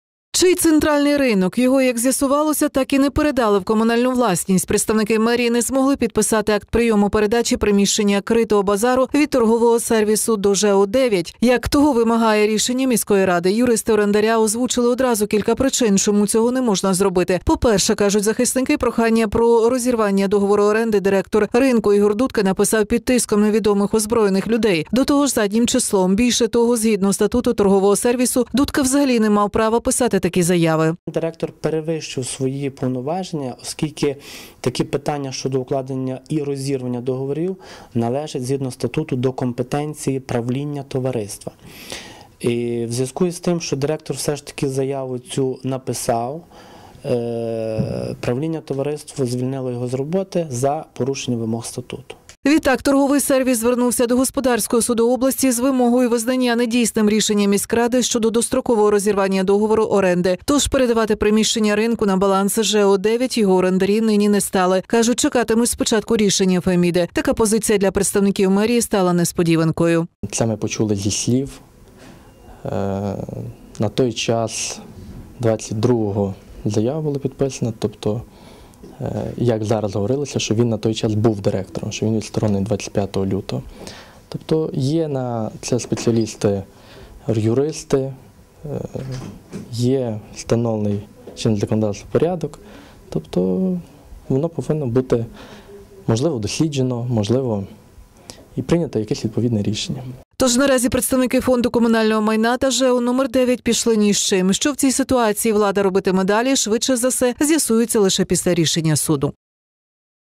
Центральний ринок у комунальну власність скоро не перейде
Чий центральний ринок? Його, як з'ясувалося так і не передали в комунальну власність.
Канал 402